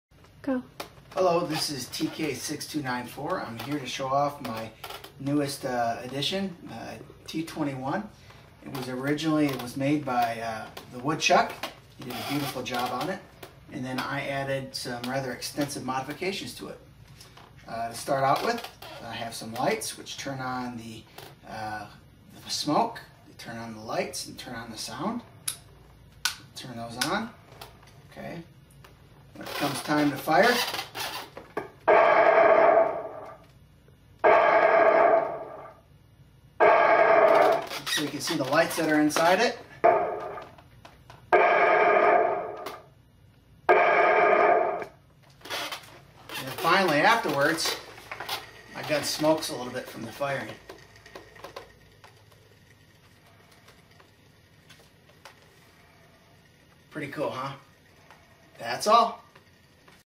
My T21 Modifications
This is a video of my newly finished T21 Lewis Gun for my Stormtrooper costume. The gun is made out of wood and was made by the Woodchuck. I then added heavy blaster sounds, background lights, firing lights, and smoke. Most impressive!